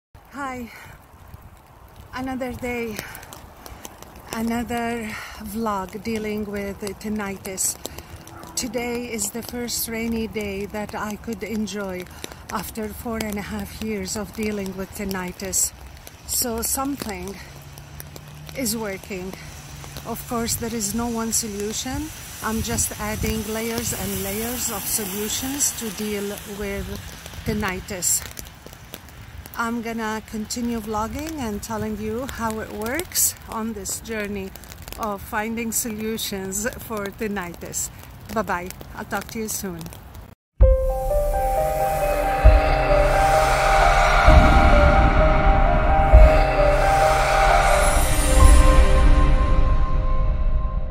Tinnitus Solutions - Another Low Tinnitus Rainy Day
Tinnitus solutions that I have tried other than conventional and alternative therapies. Well something is working.
Positive Living Because Life Matters